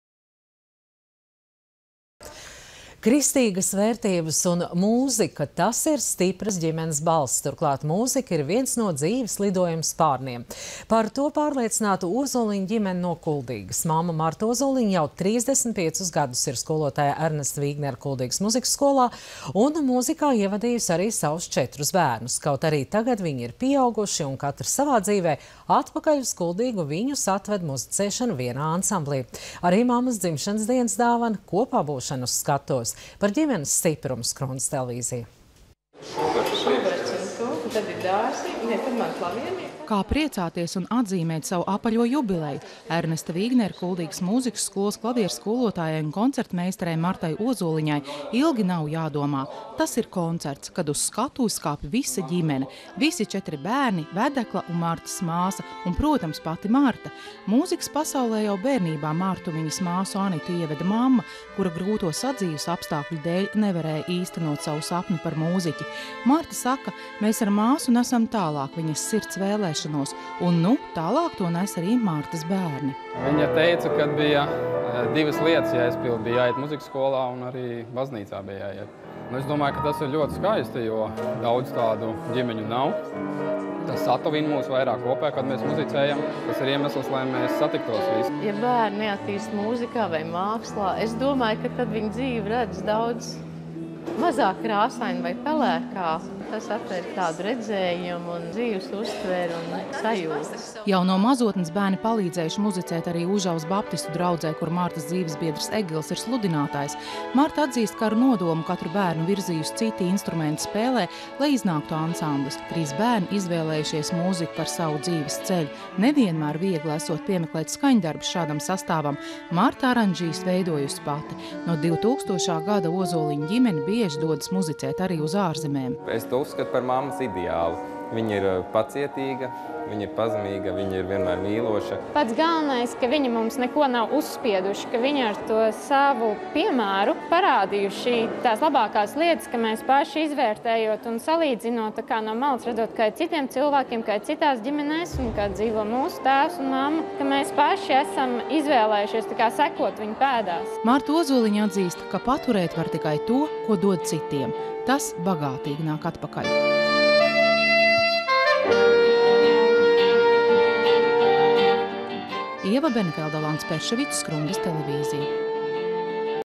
Ozoliņu ģimene no Kuldīgas muzicē kopā
Kristīgās vērtības un mūzika – tas ir stipras ģimenes balsts. Turklāt mūzika ir viens no dzīves lidojuma spārniem – par to pārliecināta Ozoliņu ģimene no Kuldīgas. Mamma Marta Ozoliņa jau 35 gadus ir skolotāja Ernesta Vīgnera Kuldīgas mūzikas skolā un mūzikā ievadījusi arī savus četrus bērnus. Kaut arī tagad viņi ir pieauguši un katrs savā dzīvē, atpakaļ uz Kuldīgu viņus atved muzicēšana vienā ansamblī. Arī mammas dzimšanas dienas dāvana – kopā būšana uz skatuves. Par ģimenes stiprumu – Skrundas TV.